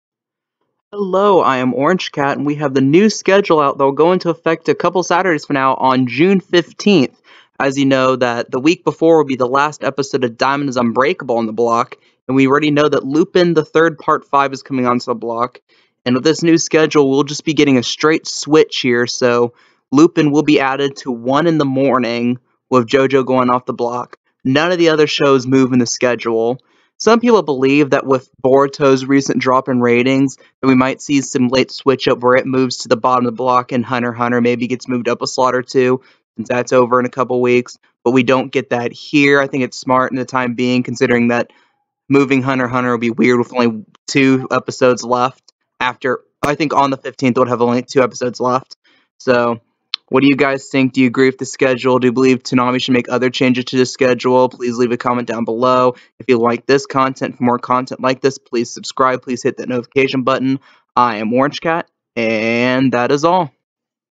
SCHEDULE UPDATE! Toonami Schedule 6/15/19
Lupin Part 5 comes onto the schedule and Jojo leaves. Overall, the changes are minimal.